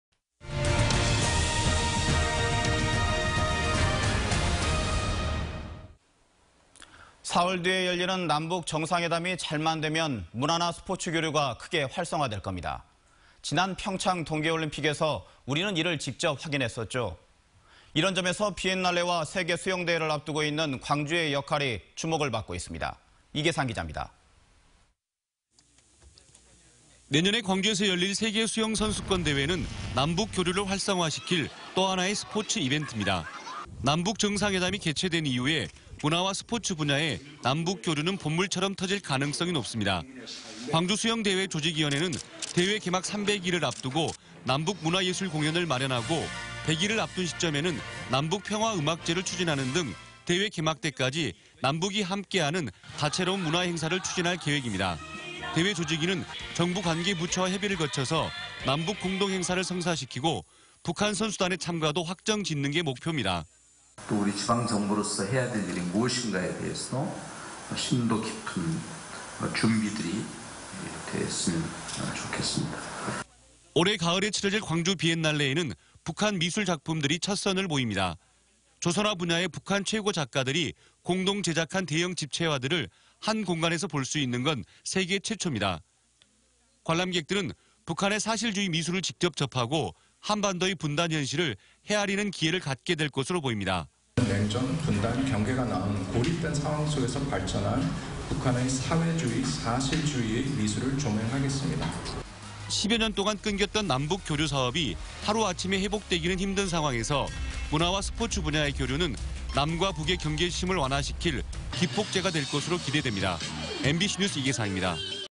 [뉴스데스크]남북 문화 스포츠 교류 기대
(앵커)

사흘 뒤에 열리는
남북 정상회담이 잘만 되면
문화나 스포츠 교류가
크게 활성화 될 겁니다.

지난 평창 동계올림픽에서
우리는 이를 직접 확인했는데요.

이런 점에서 비엔날레와
세계 수영대회를 앞두고 있는
광주의 역할이
주목을 받고 있습니다.

이계상 기자입니다.

(기자)
내년에 광주에서 열릴 세계수영선수권대회는
남북 교류를 활성화시킬
또 하나의 스포츠 이벤트-ㅂ니다.

남북 정상회담이 개최된 이후에
문화와 스포츠 분야의
남북 교류는 봇물처럼 터질 가능성이 높습니다.

광주수영대회 조직위원회는
대회 개막 3백일 앞두고
남북 문화예술 공연을 마련하고,

백일을 앞둔 시점에는
남북 평화음악제를 추진하는 등
대회 개막때까지 남북이 함께 하는
다채로운 문화행사를 추진할 계획입니다.

대회 조직위는
정부 관계 부처와 협의를 거쳐
남북 공동 행사를 성사시키고
북한 선수단의 참가도 확정짓는 게 목푭니다.

(인터뷰)-윤장현
'지방 정부 차원의 할 수있는 일 다 하자'

올해 가을에 치러질 광주비엔날레에는
북한 미술 작품들이 첫 선을 보입니다.

조선화 분야의 북한 최고 작가들이
공동 제작한 대형 집체화들을
한공간에서 볼 수 있는 건 세계 최초-ㅂ니다.

관람객들은
북한의 사실주의 미술을 직접 접하고
한반도의 분단 현실을
헤아리는 기회를 갖게될 것으로 보입니다.

(현장녹취)-'북한 사실주의 미술 재조명 기회'

10여년 동안 끊겼던 남북 교류사업이
하루 아침에 회복되기는 힘든 상황에서

문화와 스포츠 분야의 교류는
남과 북의 경계심을 완화시킬
기폭제가 될 것으로 기대됩니다.